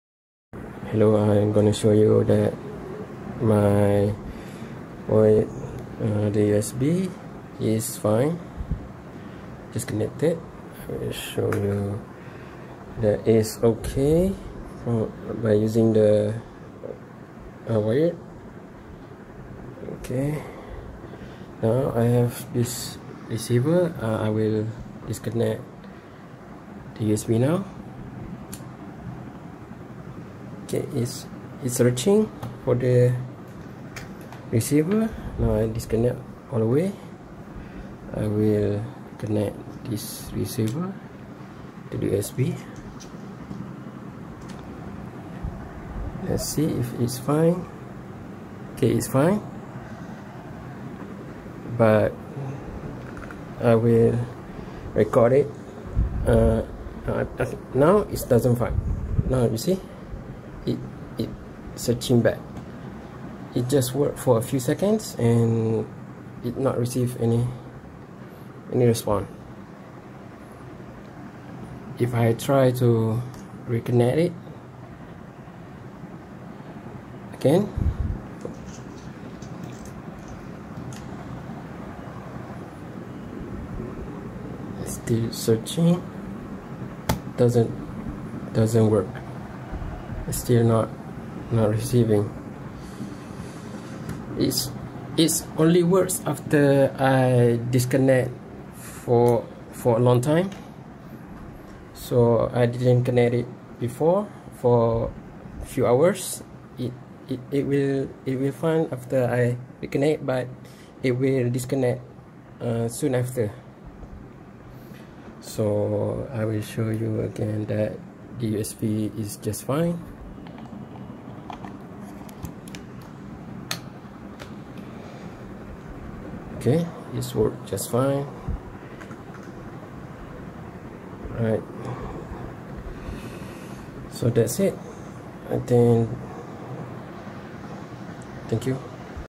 Razer Viper V2 pro dongle not working..
This video to demonstrate and proof for the razer support.